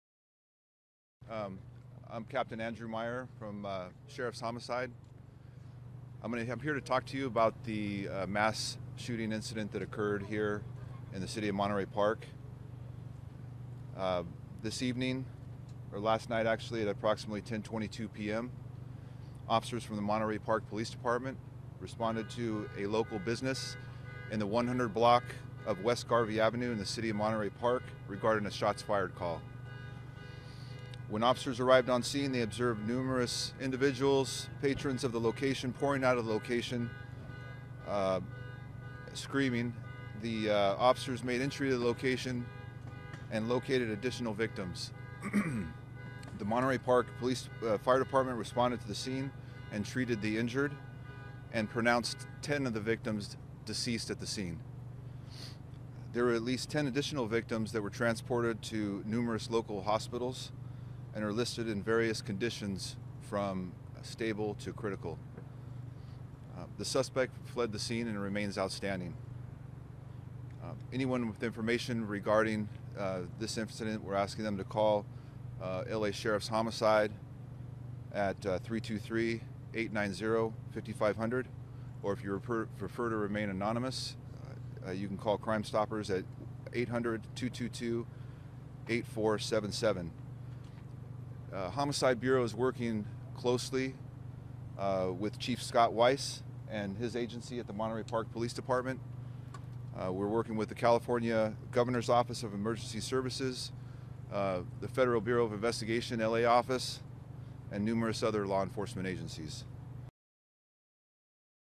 Gunman Opens Fire inside Dace Club in Monterey Park Mass Shooting
(AP) - A gunman killed 10 people and wounded 10 others at a Los Angeles-area ballroom dance club following a Lunar New Year celebration, setting off a manhunt for the suspect in the latest mass shooting tragedy in an American community.

Capt. Andrew Meyer of the Los Angeles Sheriff’s Department said Sunday that the wounded were taken to hospitals and their conditions range from stable to critical. He said the 10 people died at the scene in the city of Monterey Park.

Meyer said people were “pouring out of the location screaming” when officers arrived at around 10:30 p.m. Saturday. He said officers then went into the ballroom and found victims as firefighters treated the wounded.

Meyer gave no description of the male suspect or the weapon he used, or why police gave no information on the shooting for hours while the shooter remained on the run. He also said police were investigating another incident in the nearby city of Alhambra to see whether it was connected.

Meyer said it’s too early in the investigation to know if the gunman knew anyone at the ballroom or if it was a hate crime or not. He gave no other details.

The Lunar New Year celebration had attracted thousands. Monterey Park is a city of about 60,000 people with a large Asian population about 10 miles (16 kilometers) from downtown Los Angeles. Two days of festivities were planned but officials cancelled Sunday's events following the shooting.